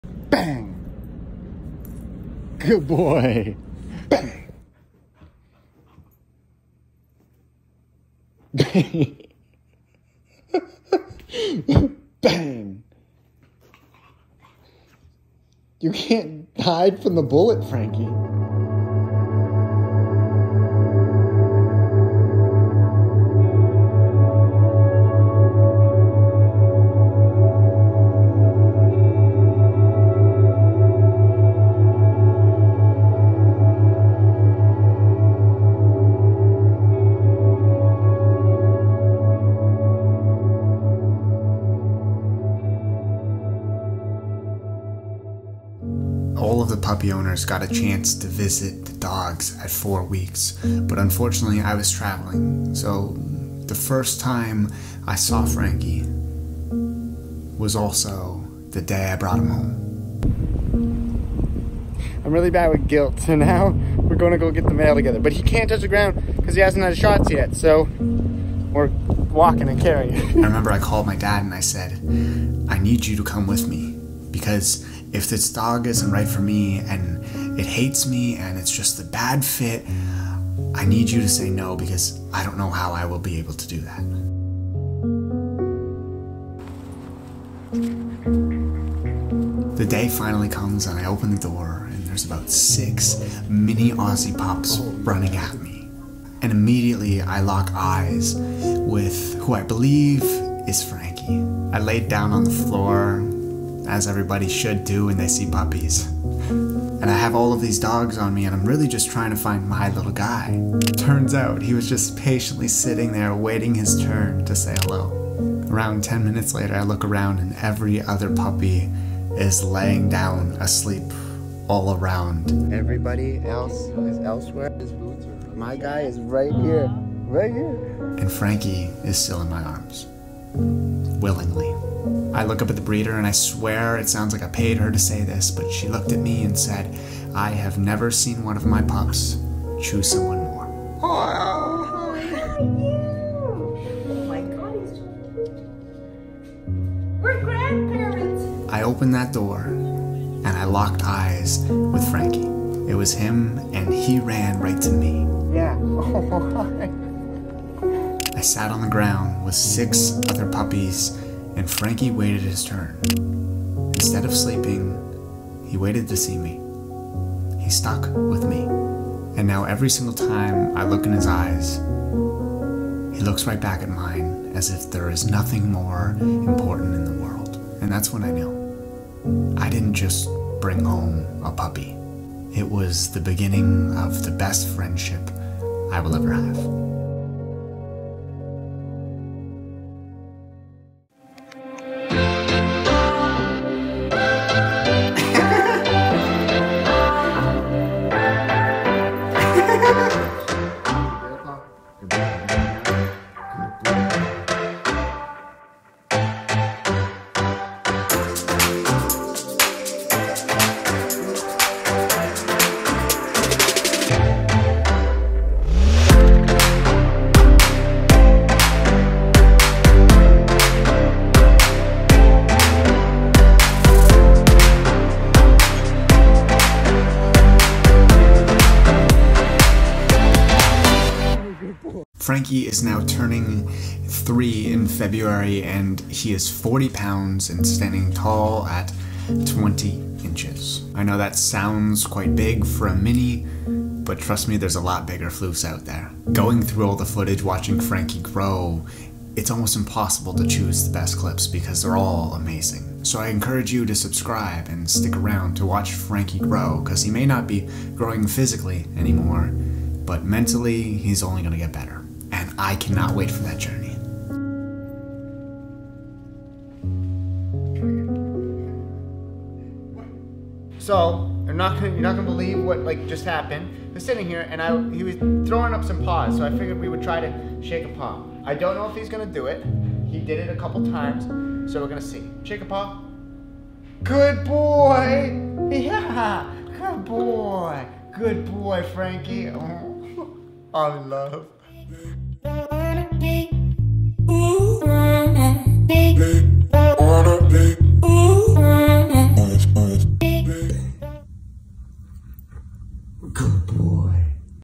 Watch My Puppy Grow | Mini Australian Shepherd
➳ It is time for everyone to get puppy fever! I thought this would be a good video to watch back and see how Frankie grew over the last few years. If you are interested, I can make a video going into more detail about how the process went.

» Water Bottle
» Kong Frisbee
» Poop Bags
» Car Seatbelt